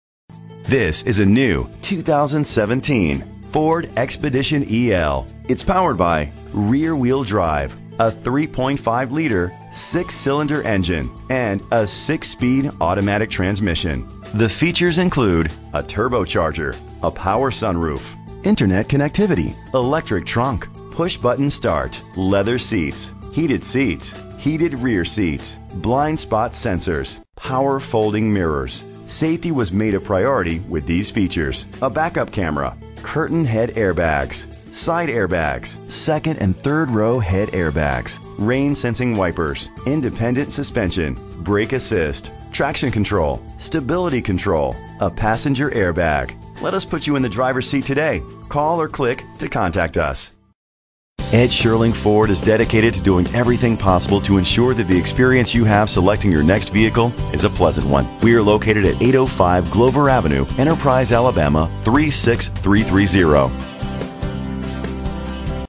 2017 Ford Expedition EL - Enterprise AL
Phone:
Year: 2017
Make: Ford
Model: Expedition EL
Trim: Limited
Engine: 3.5 liter 6 cylinder 24 valve
Transmission: 6-Speed Automatic
Color: White Platinum Metallic Tri-Coat
Mileage: 0
Address:
805 Glover Avenue
Enterprise, AL 36330